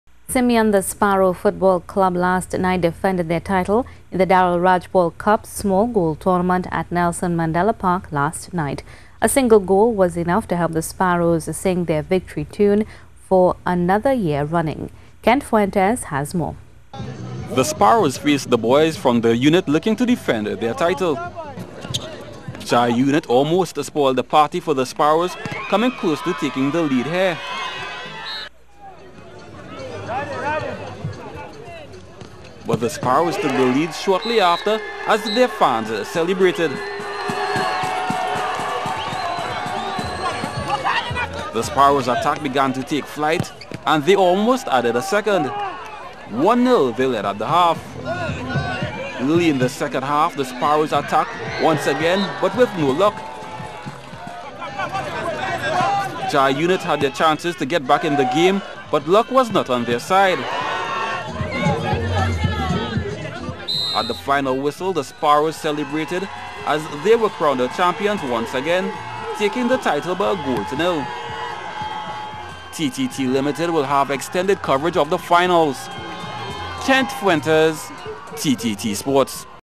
SPORT: Sparrows Defended Their Title At The Darryl Rajpaul Cup Small Goal Tournament
Simeon the Sparrow Football Club on Saturday night defended their title in the Darryl Rajpaul Cup Small Goal Tournament at Nelson Mandela Park.

A single goal was enough to help the Sparrow's sing their victory tune for another year running.